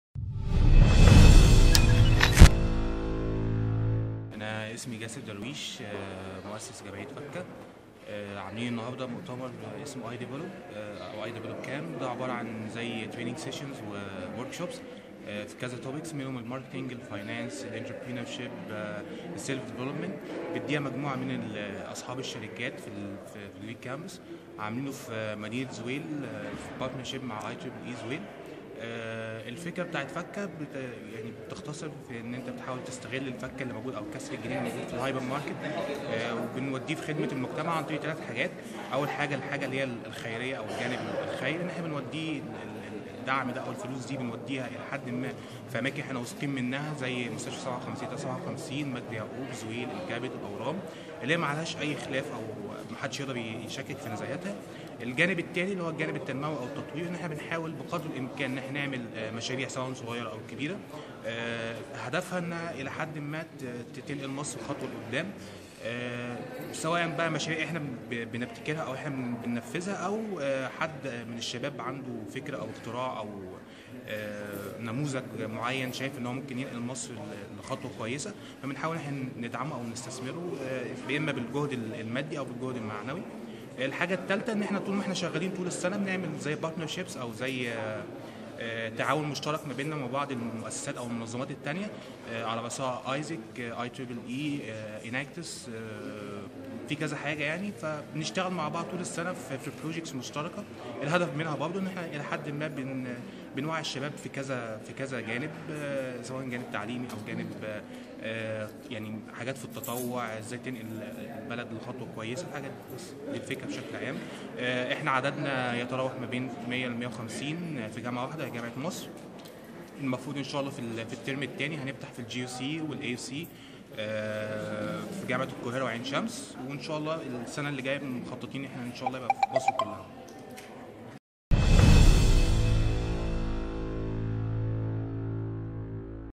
Report of iDevelop Camp on Video Youm7
Check out the interview with Mr. Gasser Darwish, Fakkah's founder talking about Fakkah and iDevelop Camp done by Video Youm7